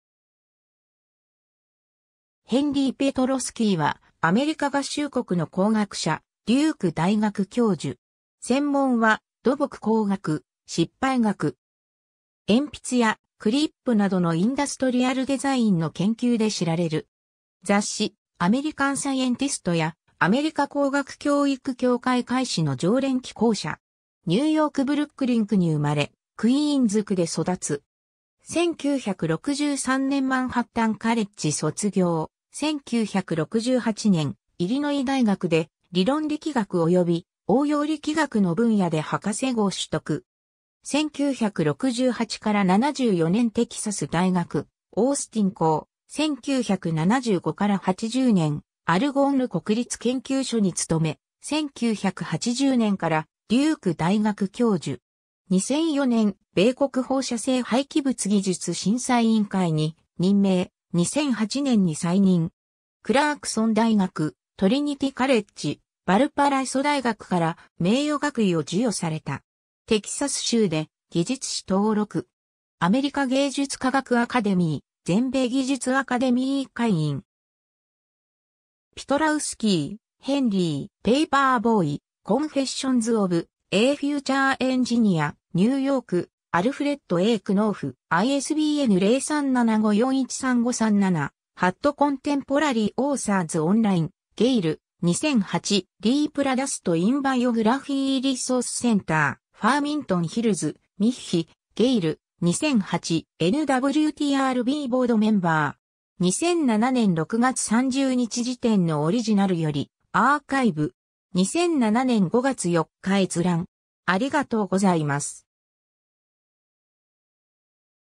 ヘンリー・ペトロスキー
ヘンリー・ペトロスキー（Henry Petroski, 1942年2月6日 - ）は、アメリカ合衆国の工学者、デューク大学教授。
専門は土木工学、失敗学。
 鉛筆やクリップなどのインダストリアルデザインの研究で知られる。
雑誌『アメリカン・サイエンティスト』 (en) やアメリカ工学教育協会会誌 (en) の常連寄稿者。
 ニューヨーク・ブルックリン区に生まれ、クイーンズ区で育つ。
1963年マンハッタン・カレッジ卒業、1968年イリノイ大学で理論力学および応用力学の分野で博士号取得。
1968-74年テキサス大学オースティン校、1975-80年アルゴンヌ国立研究所に勤め、1980年からデューク大学教授。
 2004年、米国放射性廃棄物技術審査委員会（United States Nuclear Waste Technical Review Board、NWTRB）に任命、2008年に再任。
 クラークソン大学 (en) 、トリニティ・カレッジ、ヴァルパライソ大学 (en) から名誉学位を授与された。
テキサス州で技術士登録。
アメリカ芸術科学アカデミー、全米技術アカデミー会員。
 ^ Petroski, Henry (2002). Paperboy: Confessions of a Future Engineer. New York: Alfred A. Knopf. ISBN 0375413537 ^ Contemporary Authors Online, Gale, 2008. Reproduced in Biography Resource Center. Farmington Hills, Mich.: Gale, 2008. ^ “NWTRB Board Member”. 2007年6月30日時点のオリジナルよりアーカイブ。
2007年5月4日閲覧。